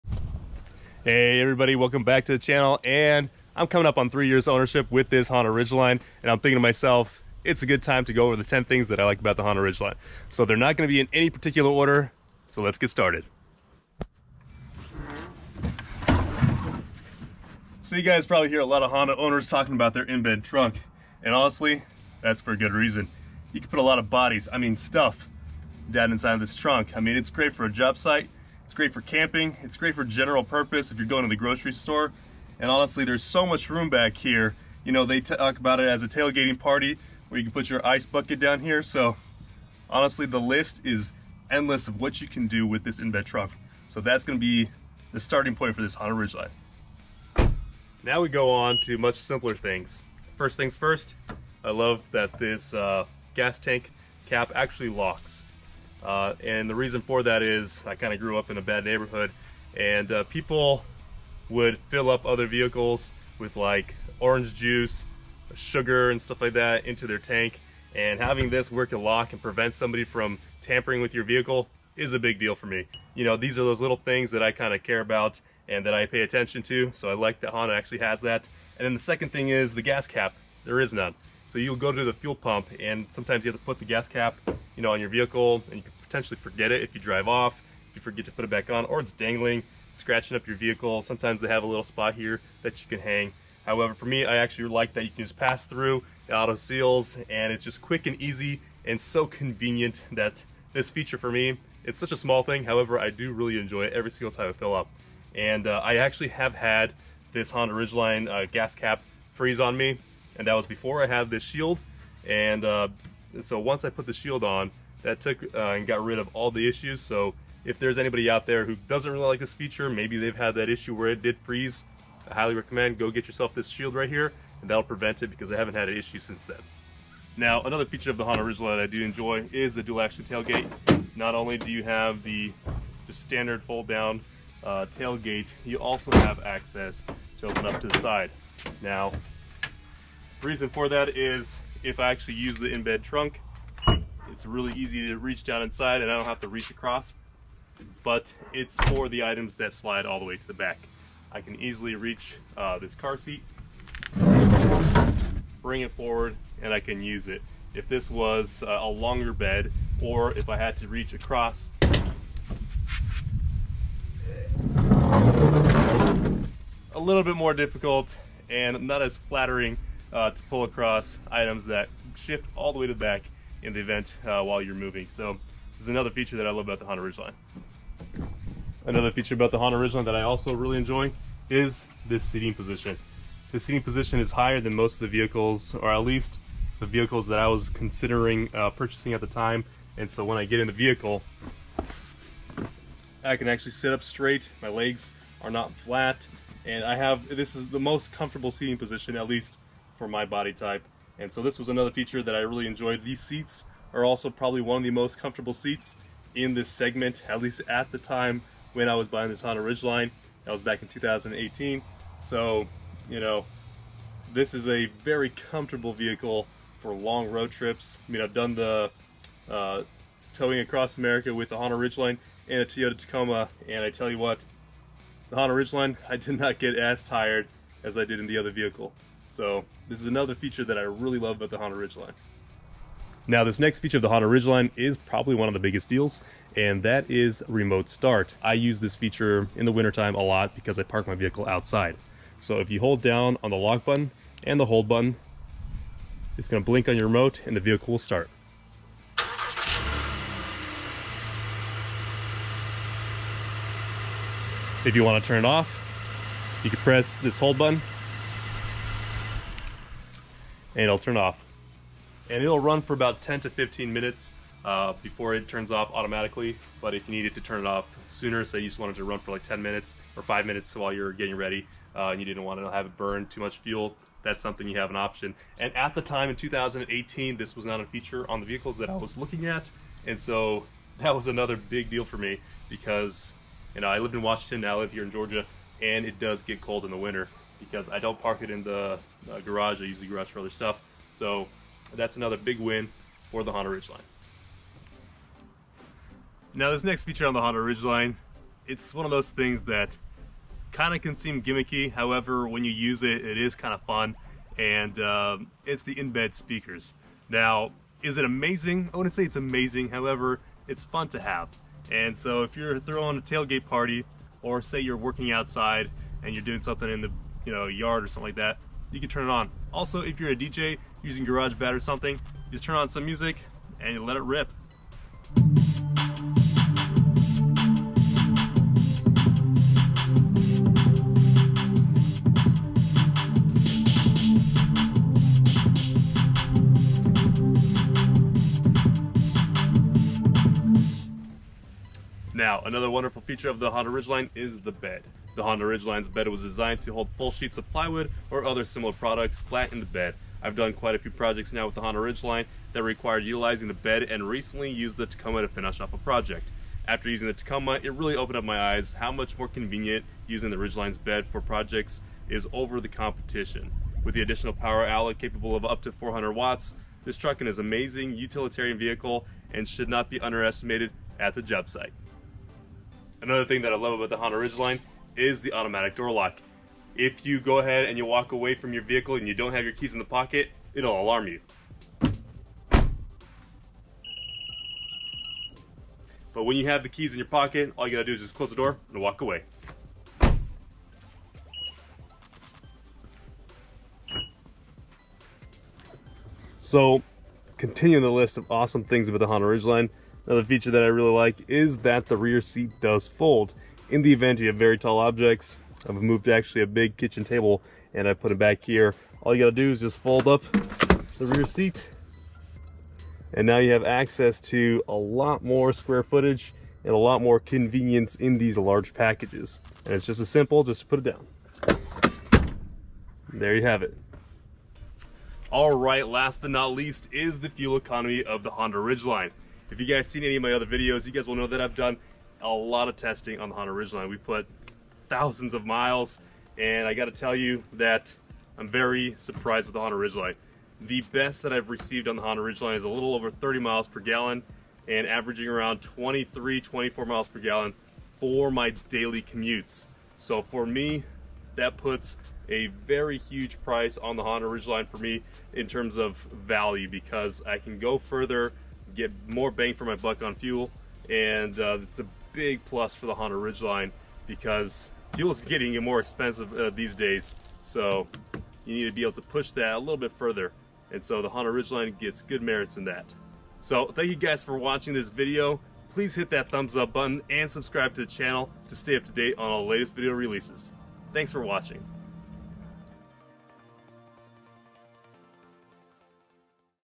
10 things I like about the Honda Ridgeline
After 3 years of ownership I have decided to go over the features that I enjoy about the 2018 Ridgeline RTL-E. They are not in any particular order.

Ridgeline Runners
Air Filter
Fuel Dust Cover
Pod Lights
Brake Light strip
Floor Mats rear
Floor Mats Front
Tonneau cover